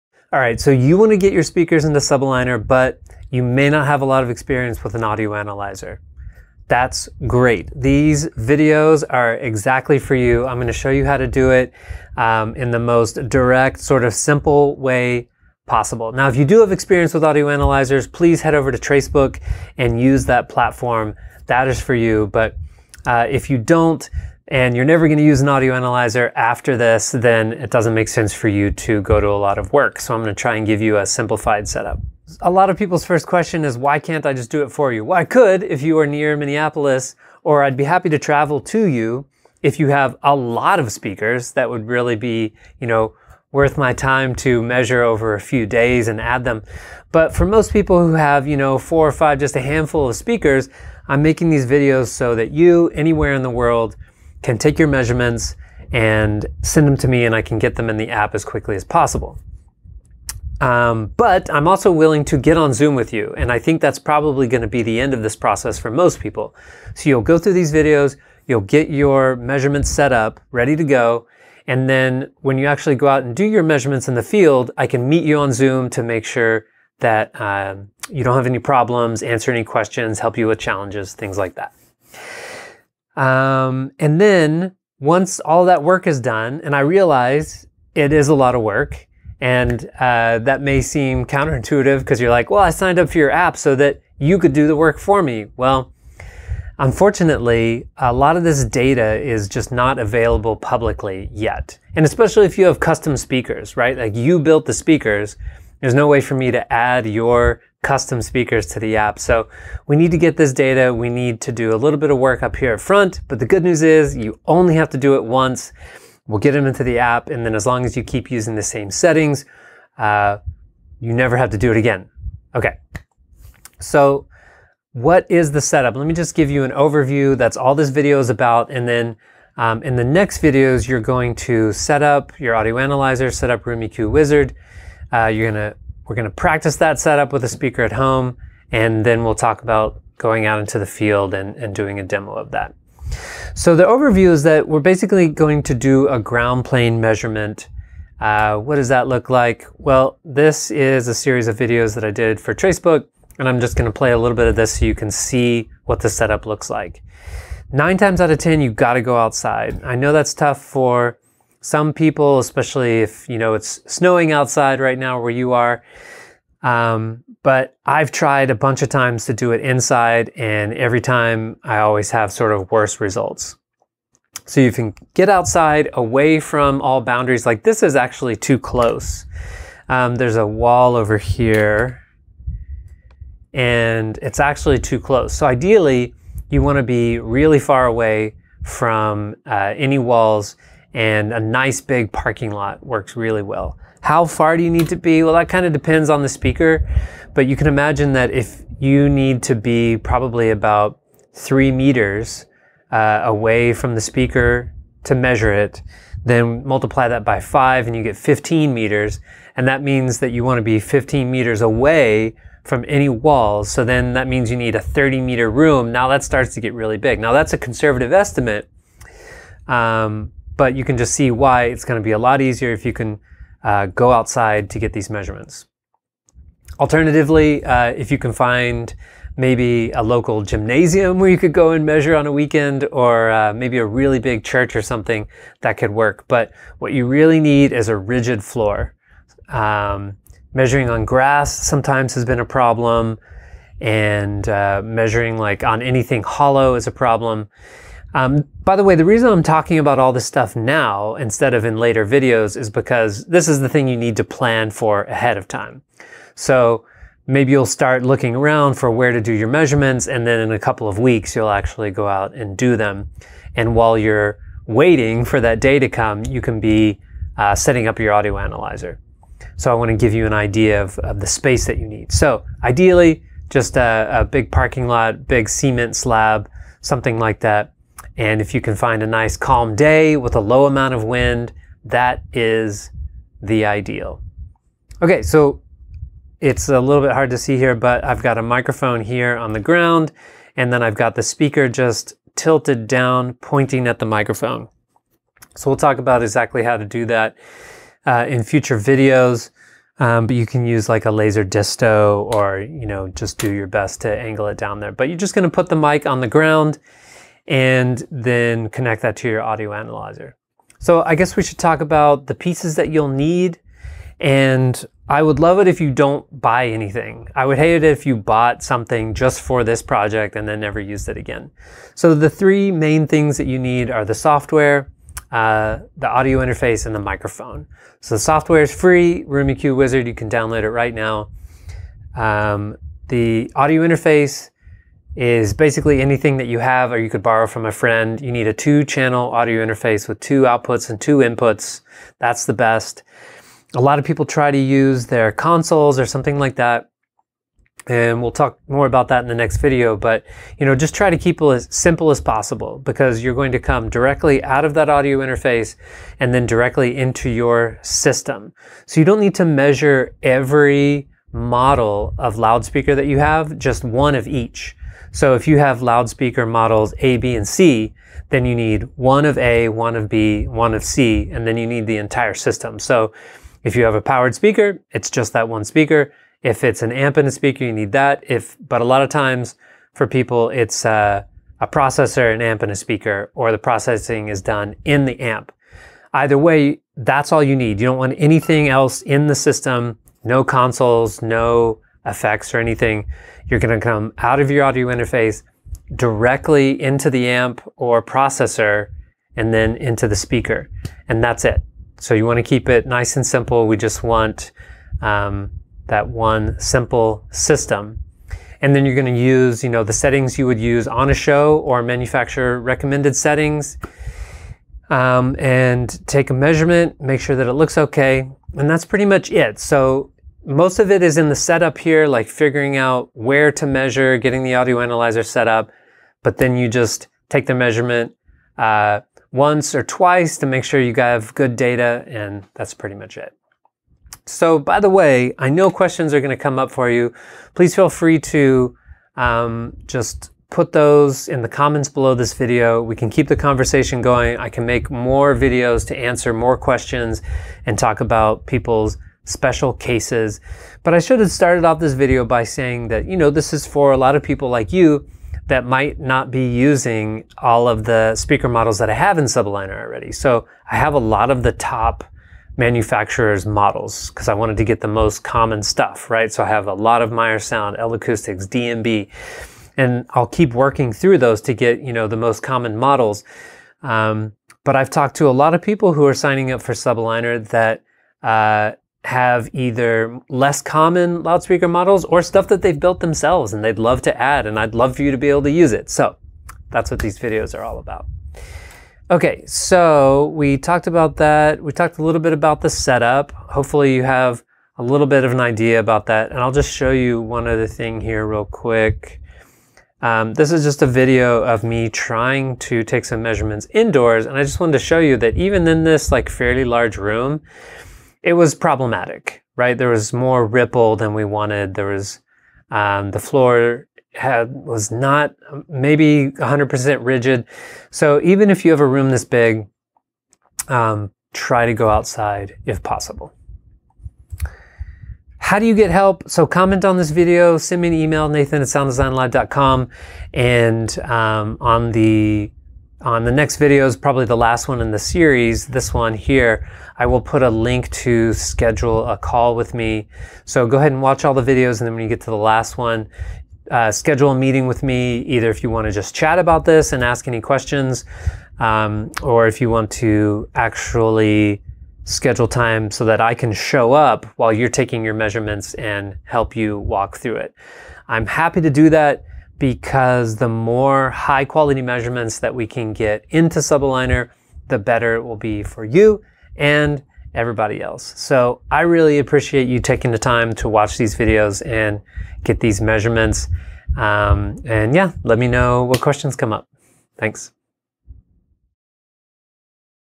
Add your speakers to SubAligner: Planning
Setup: Outdoors, if possible. Far away from boundaries. Speaker and mic on the ground. Aim speaker down at mic. Connect your audio interface directly to amp or speaker.

Equipment: Room EQ Wizard, 2ch audio interface, measurement microphone (or similar flat response condenser mic)